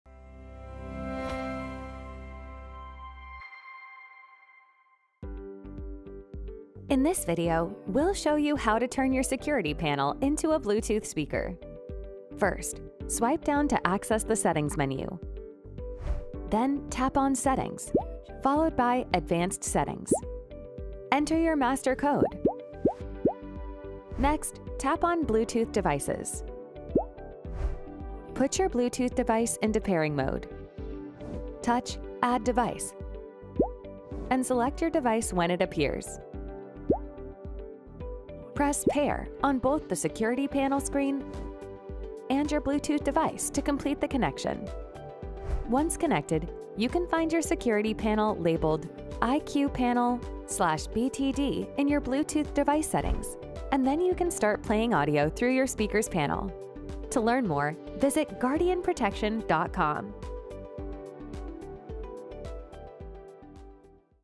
Bluetooth Speaker | Guardian Protection
Check out this video to find out how you can make your Guardian Protection security panel into a Bluetooth speaker!